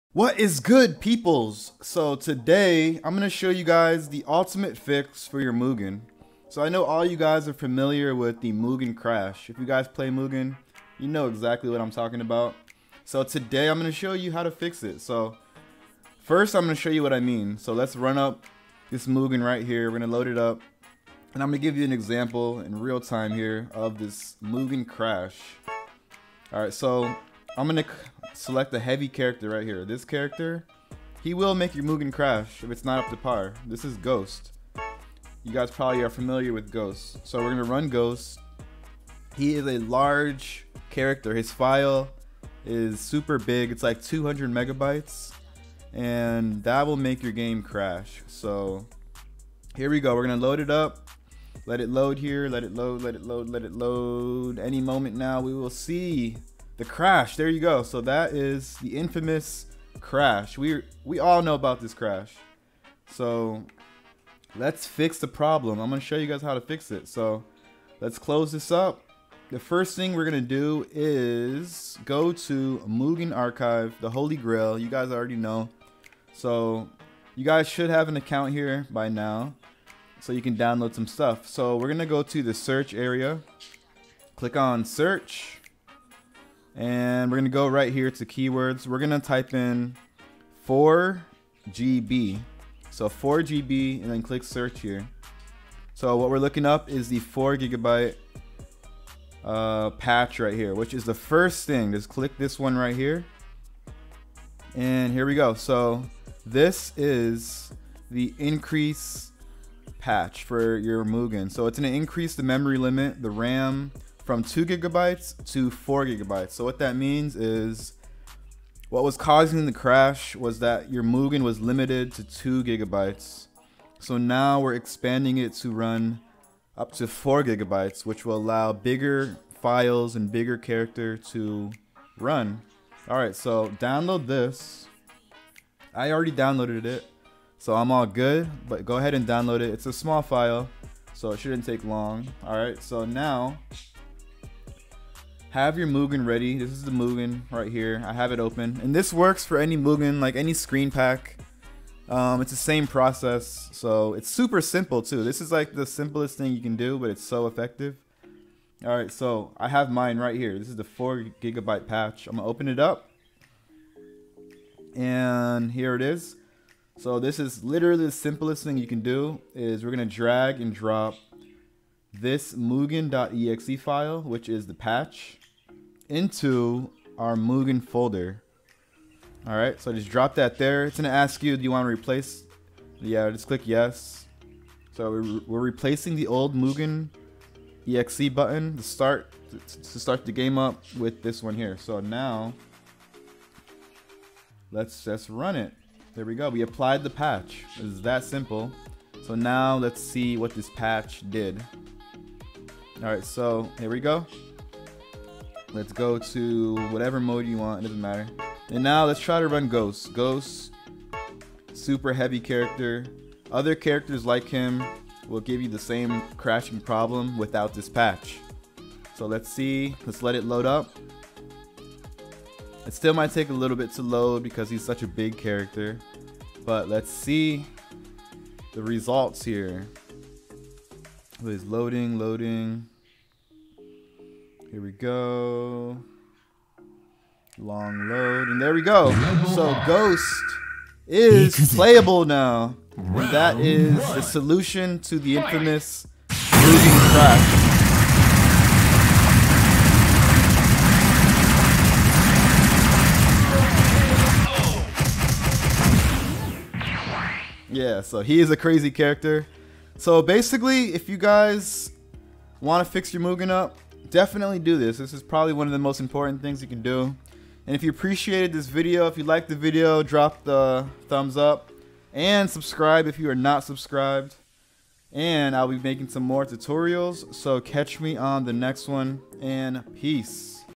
MUGEN | ULTIMATE Crash Fix Tutorial [Super Easy]
(PATCH LINK BELOW)
This the ultimate patch for fixing the infamous Mugen crash that we all know. This is fix alot of the crashes that happen for character selection, mainly for those big characters that use alot of memory and ram. Not 100% effective but it will be a big improvement if you experience crashes when it comes to certain characters.

Check my other videos for MORE tutorials!